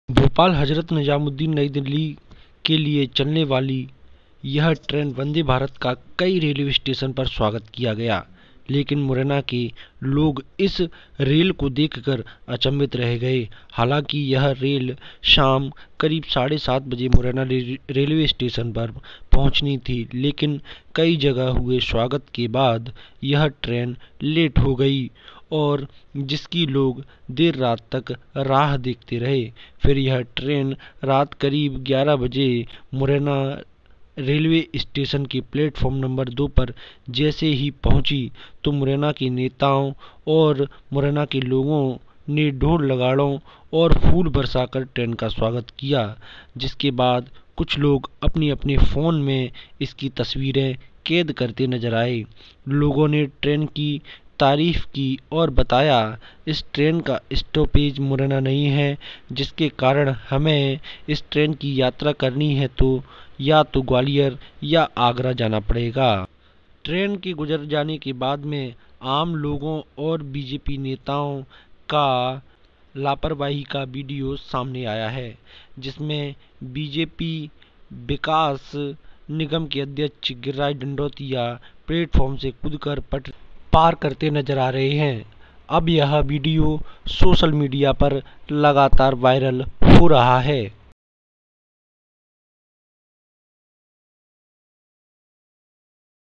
Bhopal Vande Bharat Train: वंदे भारत ट्रेन देखकर लोग उत्साहित, कहा…काश! मुरैना भी होता स्टॉपेज
लोगों ने बताया कि वंदे भारत ट्रेन भव्य और अनोखी है. इतनी सुविधाओं से पूर्ण ट्रेन आज पहली बार हमने देखी है, पर दुख इस बात का है कि मुरैना इसका स्टॉपेज नहीं है. कुछ लोग ग्वालियर में ट्रेन को देखने अंदर घुसे थे लेकिन ट्रेन चल पड़ी तो मुरैना तक पहुंच गए.